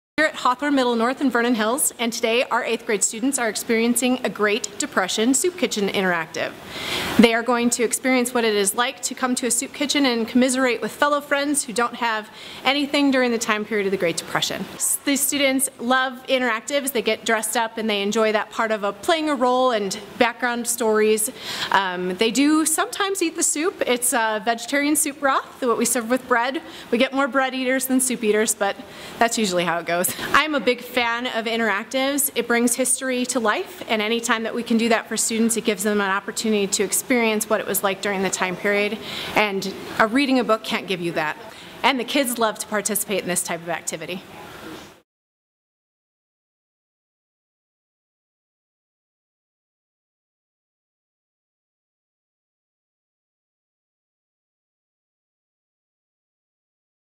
Soup kitchen serves up understanding
Eighth grade students are served soup and bread at Hawthorn Middle School North in Vernon Hills during its annual Great Depression soup kitchen.